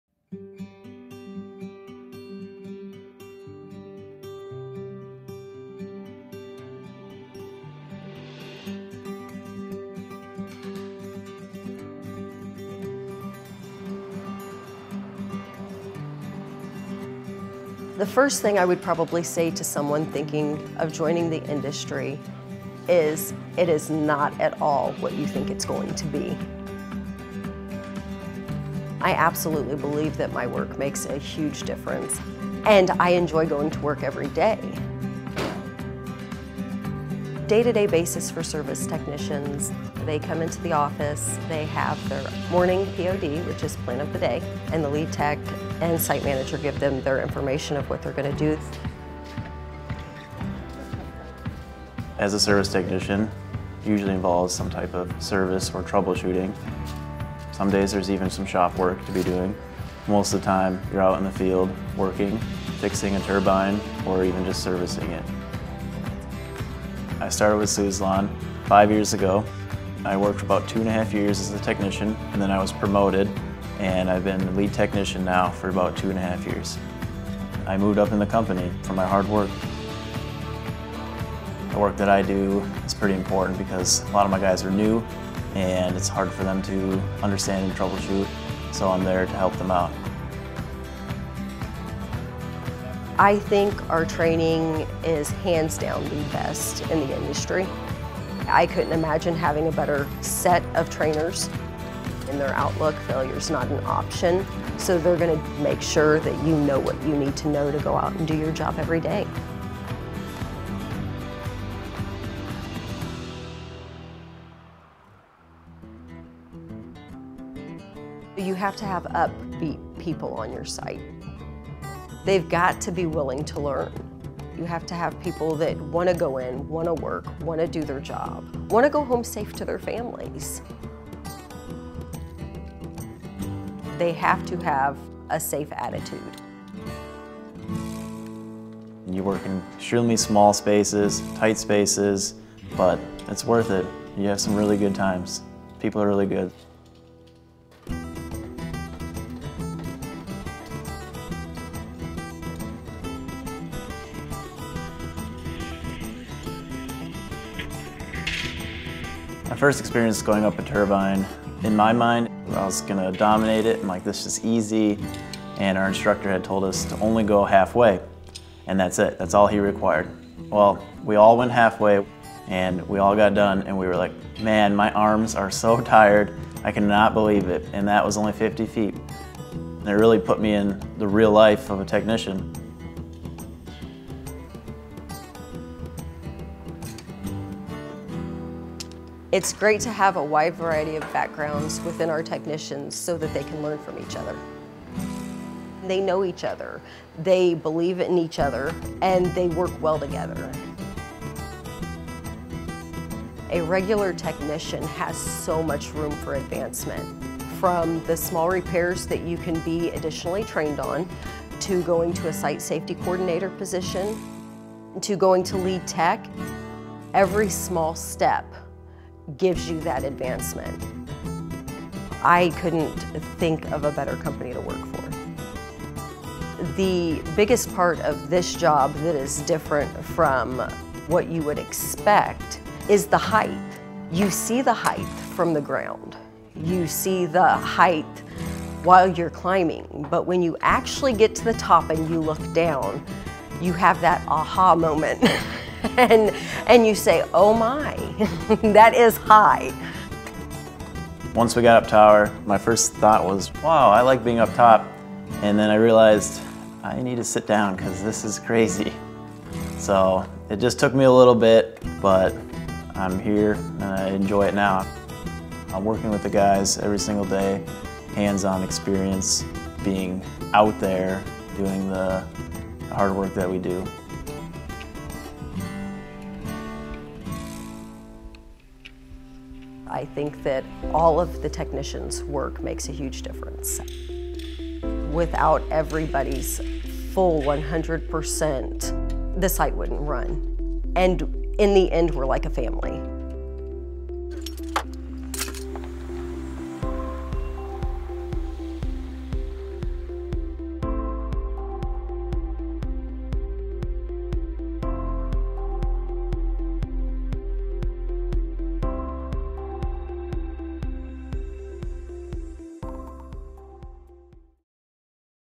A Day In The Life Of Suzlon Wind Energy Corporation Technicians
This video focuses on what the day in the life of a Suzlon Wind Energy Corporation Technician looks like. Suzlon Wind Energy Corporation operates in the United States and Canada.